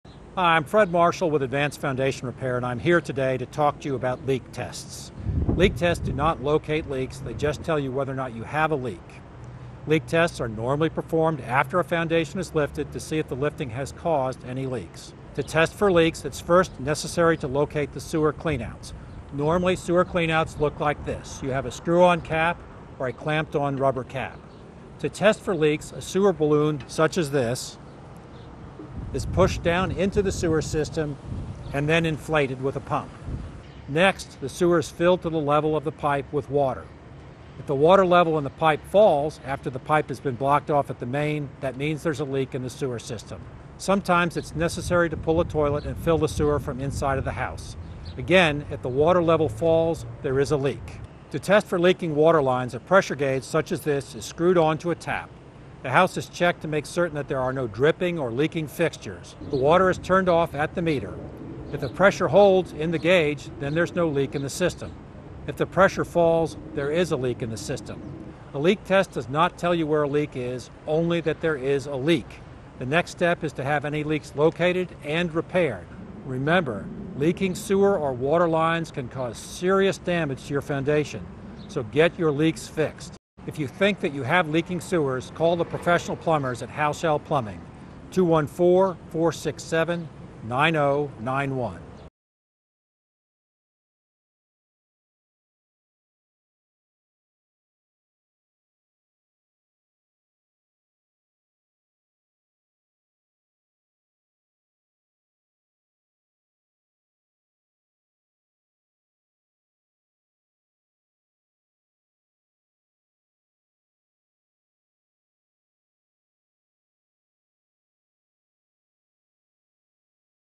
DIY Water Leak Detection Test Demo
Water leak detection test - simple step by step water leak detection check anyone can do with leak detector equipment found at any hardware store.

SCRIPT:
HI I'm Fred Marshal with Advanced Foundation Repair and I'm here today to talk to you about leak tests.
A leak test does not locate leaks.
It just tells you whether or not you have a leak.
Leak tests are normally performed after a foundation has been lifted to see if the lifting has caused any leaks.
To test for leaks it is first necessary to locate the sewer clean outs.
Normally sewer clean outs look like this.
You have a screw on cap or a clamped on rubber cap.
To test for leaks a sewer balloon such as this is pushed down into the sewer system then in-flated with a pump.
Next the sewer is filled to the level of the pipe with water.
If the water level in the pipe falls after the pipe has been blocked off at the main, that means there is a leak in the sewer system.
Sometimes it is necessary to pull a toilet and fill the sewer from inside of the house.
Again if the water level falls there is a leak.
To test for leaking water lines a pressure gage such as this is screwed onto a tap.
The house is checked to make certain there is no dripping or leaking fixtures.
The water is turned off at the meter.
If the pressure holds in the gage then there is no leak in the system.
If the pressure falls, there is a leak in the system.
  A leak test does not tell you where the leak is, only that there is a leak.
The next step is to have any leaks located and repaired.
Remember leaking sewer or water lines can cause serious damage to your foundation.
So get your leaks fixed.
If you think you have leaking sewers, call the professional plumbers at Halshall Plumbing, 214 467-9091.